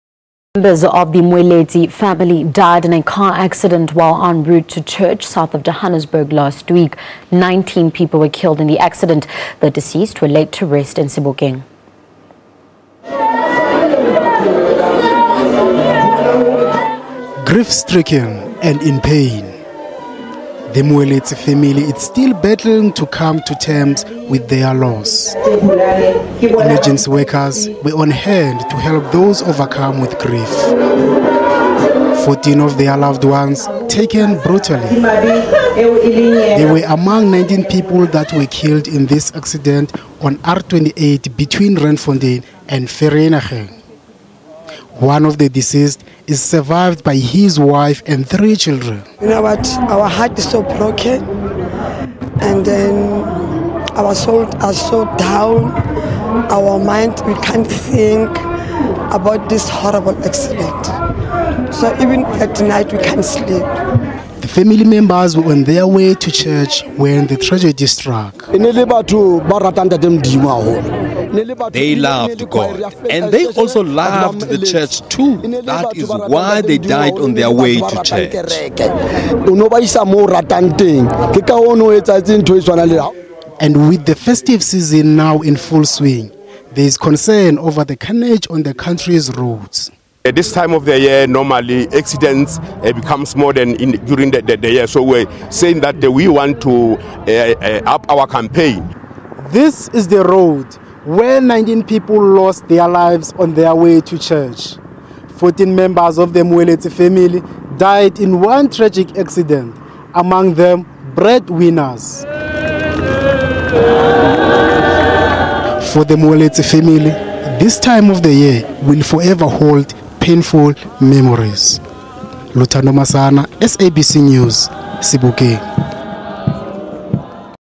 An emotional send off for 14 family members killed in an accident
A Gauteng family has described the death of fourteen members of their family in one incident as devastating.14 members of the Moeletsi family died in a car accident while en route to church south of Johannesburg last week. 19 people were killed in the accident.
The deceased were laid to rest in Sebokeng.